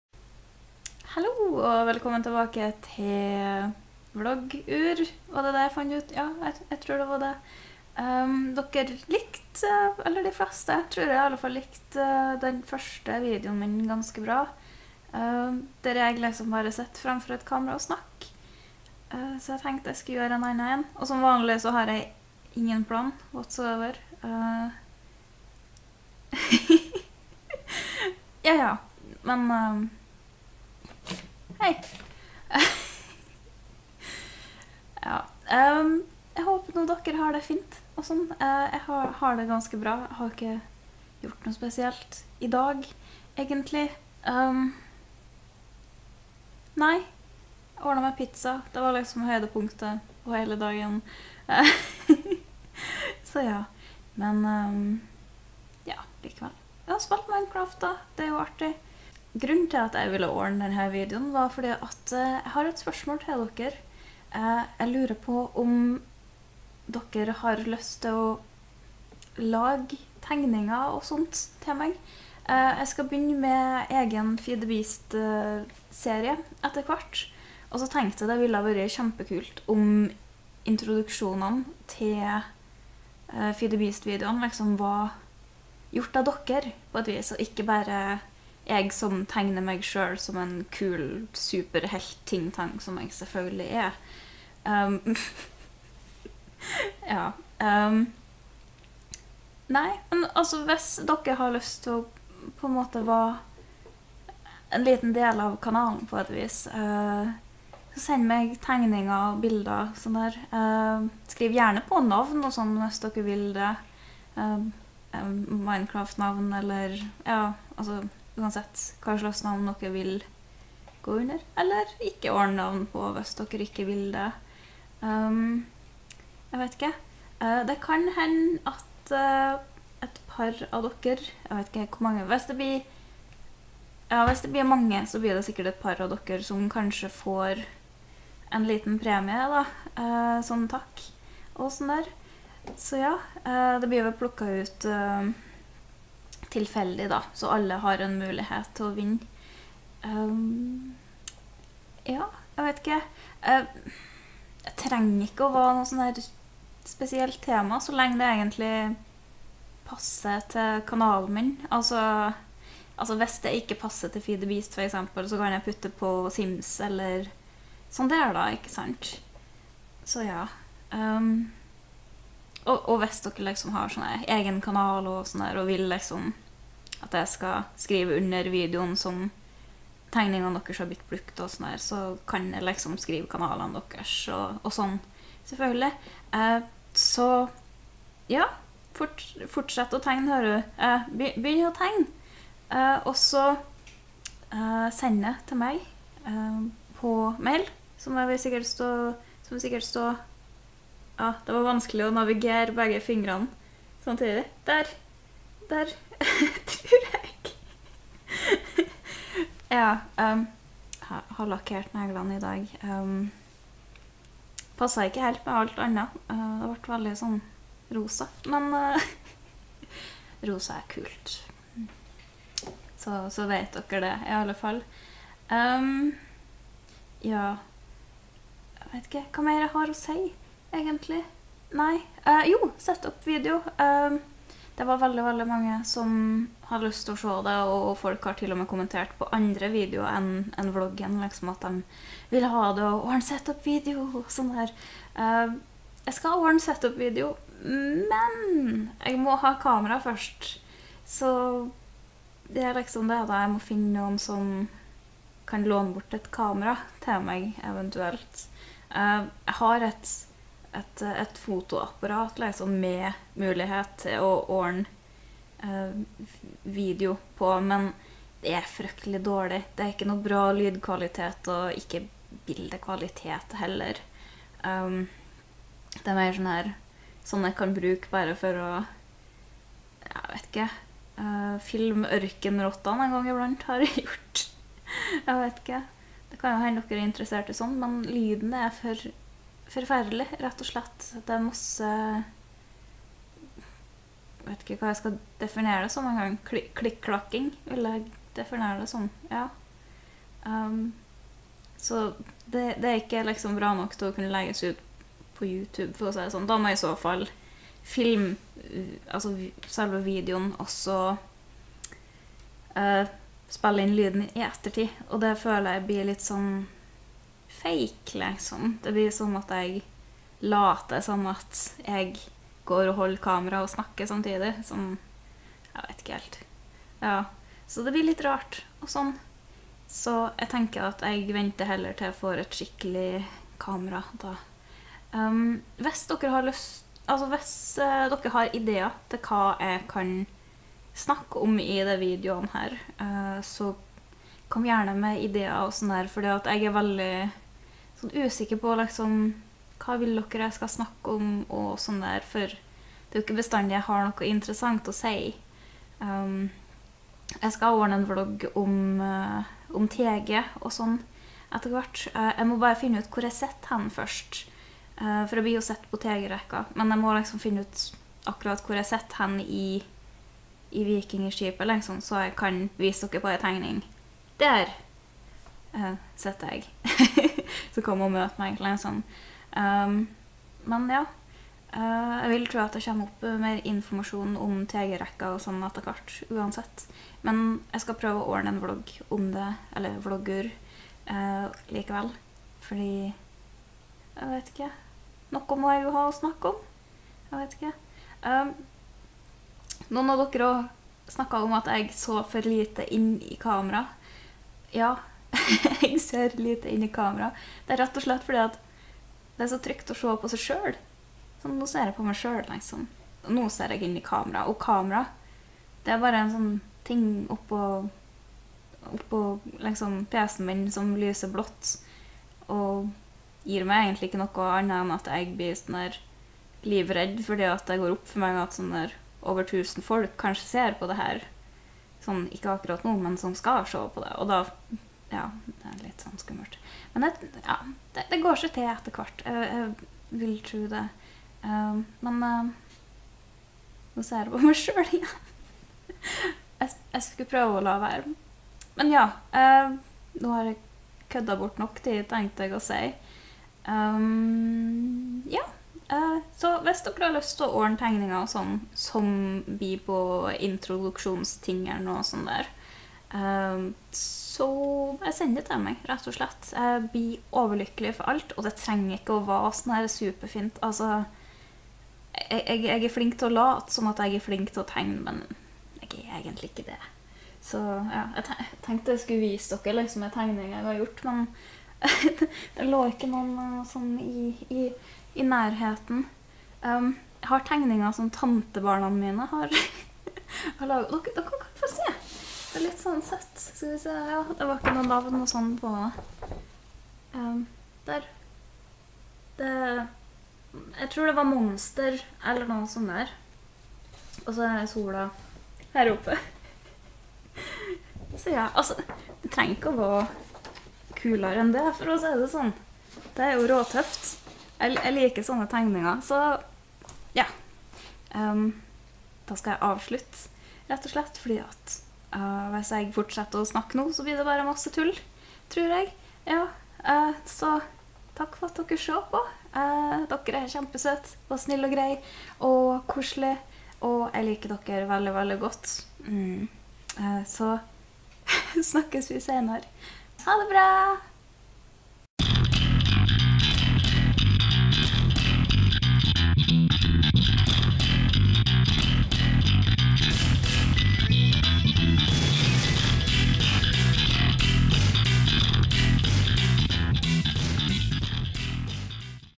Vlogur. #2: Tegning!
Vlog nummer 2, hvor jeg snakker om tegninger og random ting.